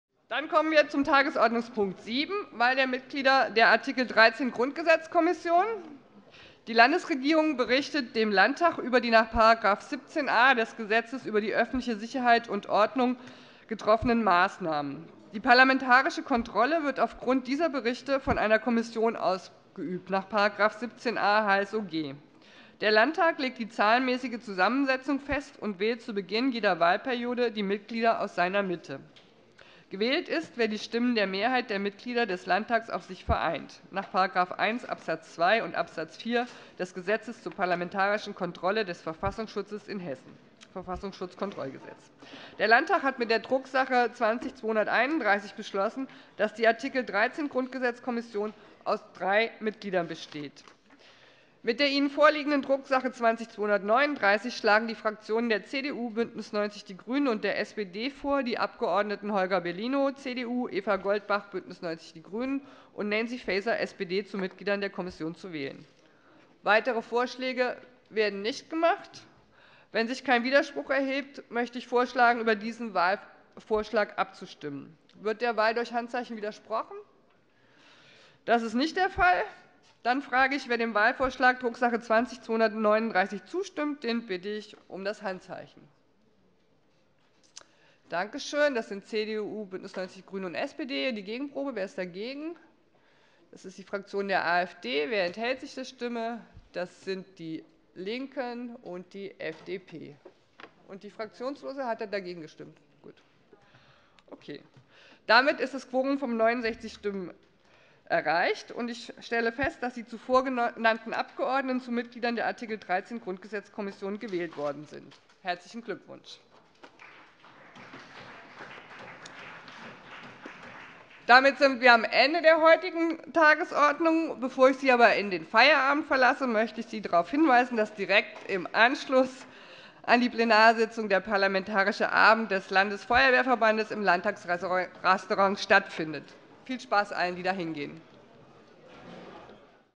Wahl Mitglieder Artikel-13-Grundgesetz-Kommission - 27.02.2019 - 6. Plenarsitzung
Wahlvorschlag Fraktion der CDU, Fraktion BÜNDNIS 90/DIE GRÜNEN, Fraktion der SPD – Drucks. 20/239